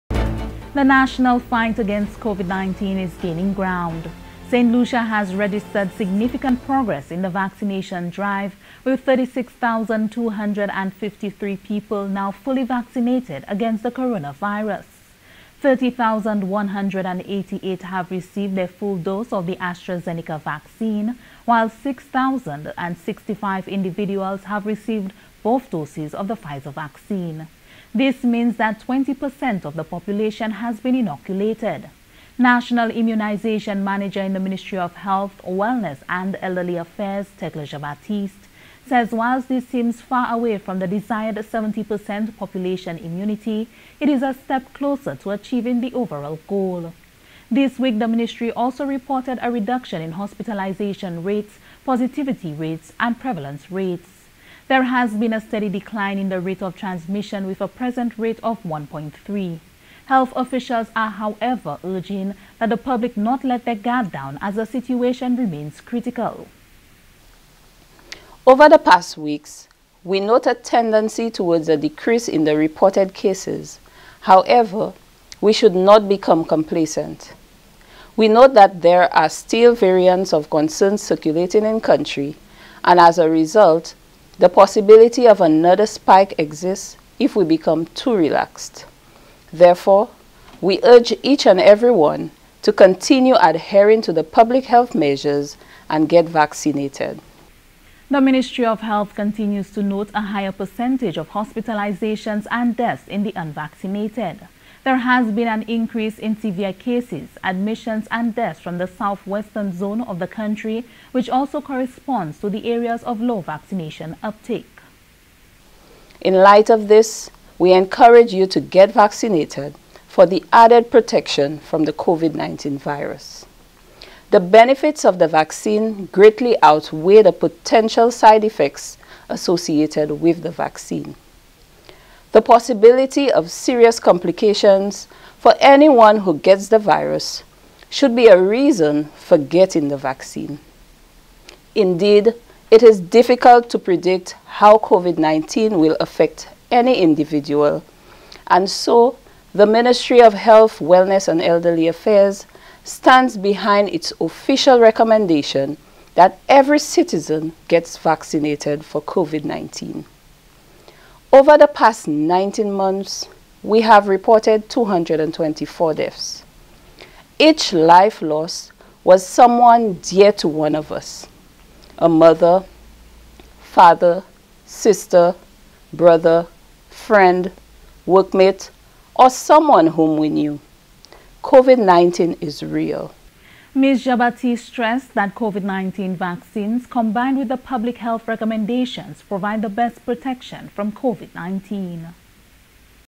Ministry of Health reports reduction in hospitalizations, COVID-19 cases
The national fight against COVID-19 is gaining ground. Saint Lucia has registered significant progress in the vaccination drive with 36, 253 people now fully vaccinated against the coronavirus.  30,188 have received their full dose of the Astra Zeneca vaccine while 6,065 individuals have received both doses of the Pfizer vaccine. This means that 20 percent of the population has been inoculated. National Immunization Manager in the Ministry of Health, Wellness and Elderly Affairs, Tecla Jn Baptiste says whilst this seems far away from the desired 70% population immunity, it is a step closer to achieving the overall goal.
This week, the Ministry also reported a reduction in hospitalization rates, positivity rates, prevalence rates. There has been steady decline in the rate of transmission with a present rate of 1.3.
Health officials are however urging that the public not let their guard down as the situation remains critical.